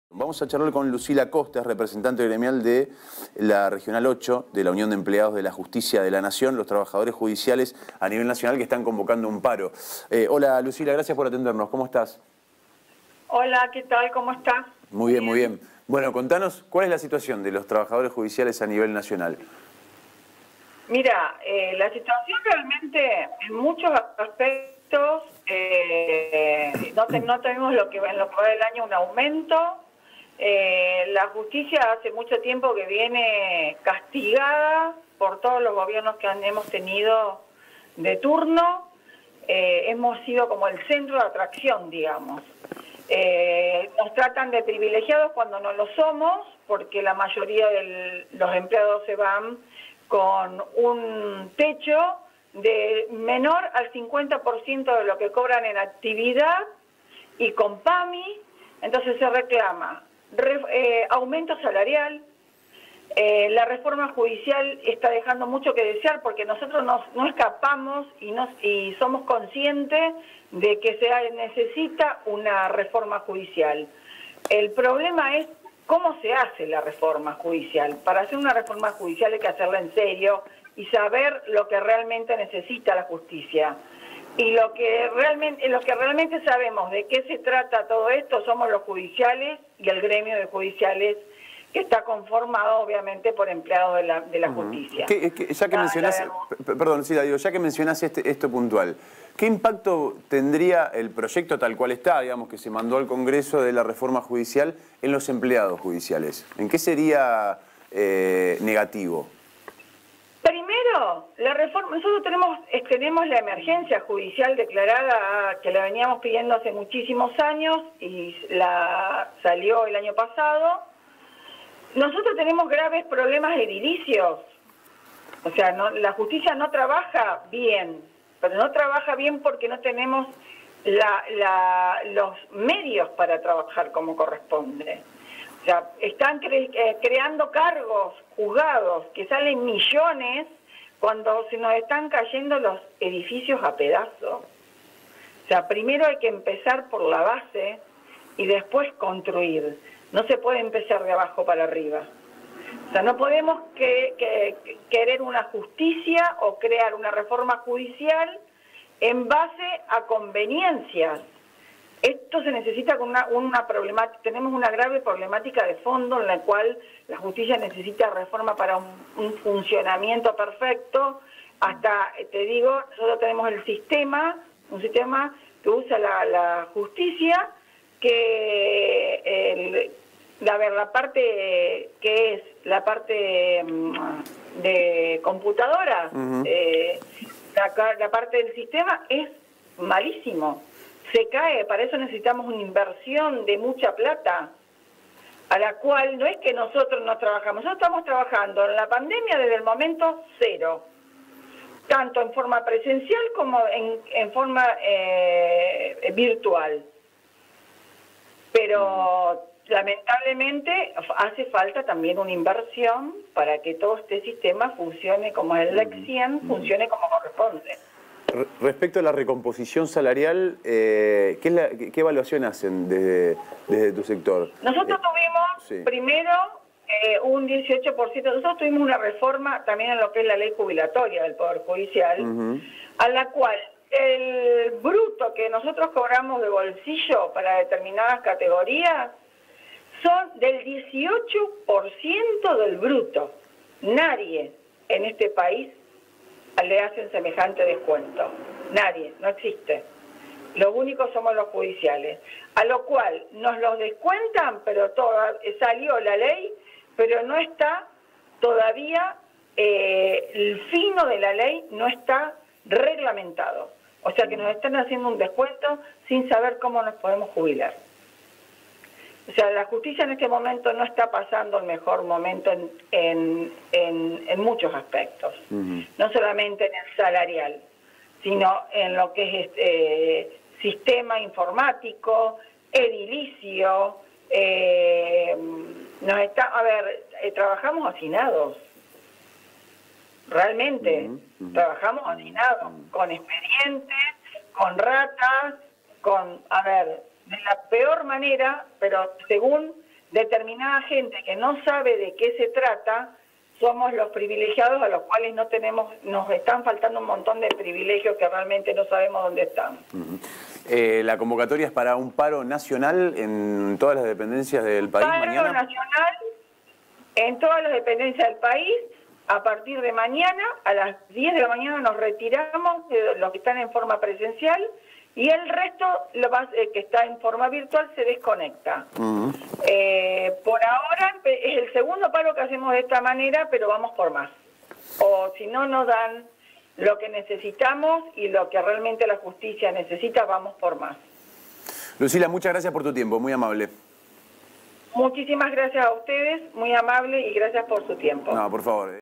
Lucila Costa - Representante gremial de la Regional 8 de la UEJN - ATP 09 09 20
La Unión de Empleados de Justicia de la Nación advirtió que si no hay respuesta en el acuerdo de ministros, llevarán adelante un cese de actividades el jueves 10 en todas las dependencias del país.

también volverán a expresar su rechazo a un aspecto de la reforma judicial, el cual plantea un eventual traspaso de la justicia penal ordinaria al ámbito de la Ciudad Autónoma de Buenos Aires.
En medio del debate en comisiones del proyecto para introducir cambios en la Justicia, Piumato expresó su disconformidad a la iniciativa del oficialismo y la calificó de "inoportuna", debido a que las prioridades deben estar puestas en la pandemia.

"Estamos en pandemia, en ninguna nación del mundo en parlamentos están discutiendo reformas judiciales, hoy las prioridades son otras. Hay que discutir necesidades imperiosas acorde a este drama", sostuvo ante los legisladores.